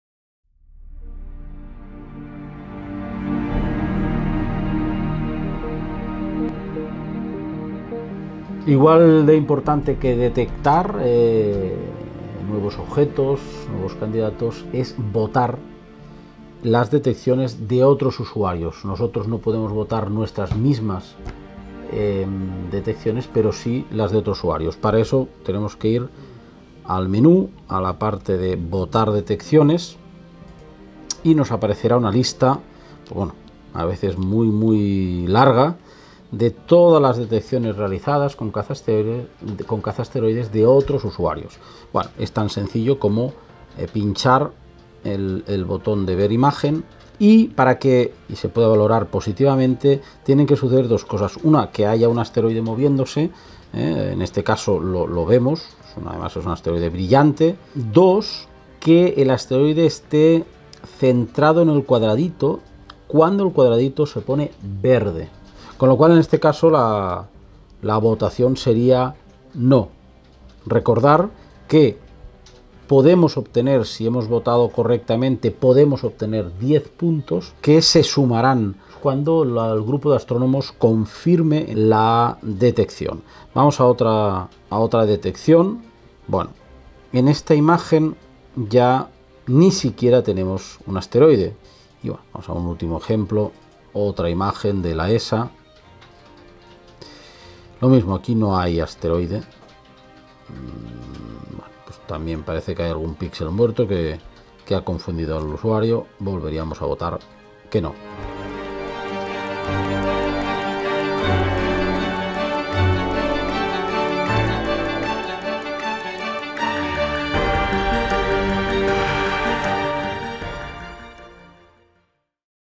CAZASTEROIDES votacion
Tutorial para Votar detecciones de Asteroides en la app Cazasteroides (cazasteroides.org)